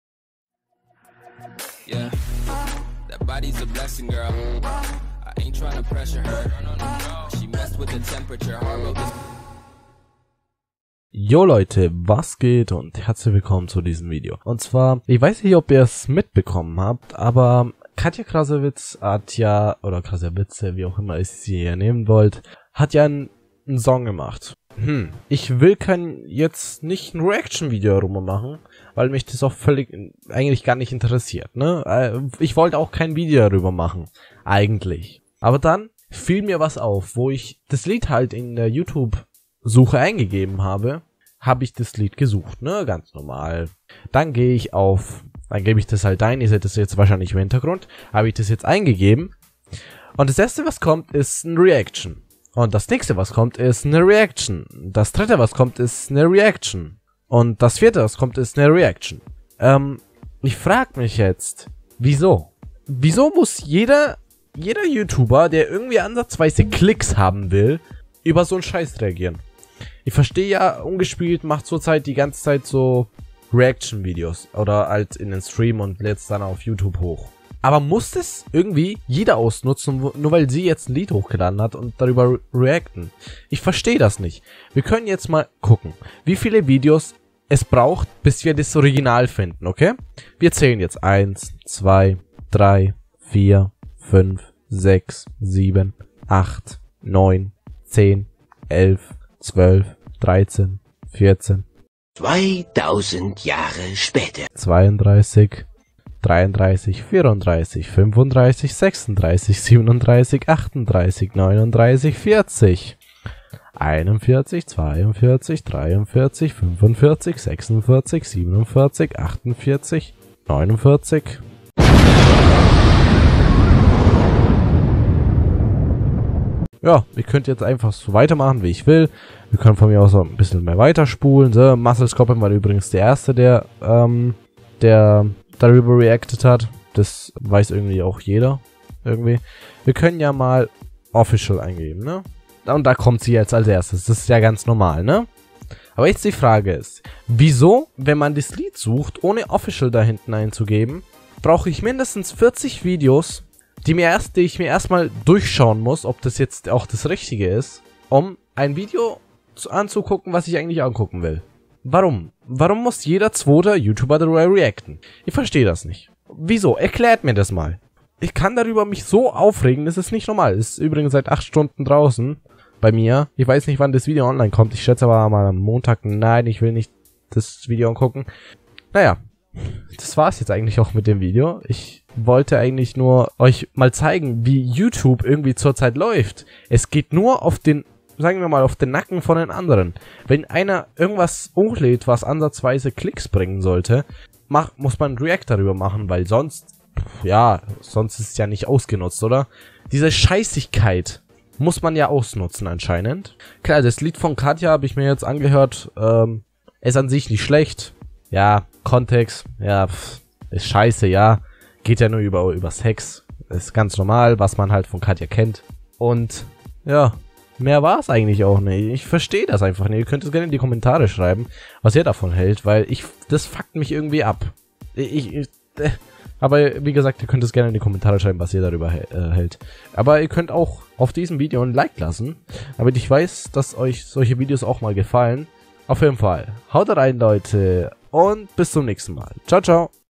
Songs Reactions ohne Ende! :(
Ein Video rund um Reactions und Behinderung der YouTube-Szene!
Es fuckt richtig ab!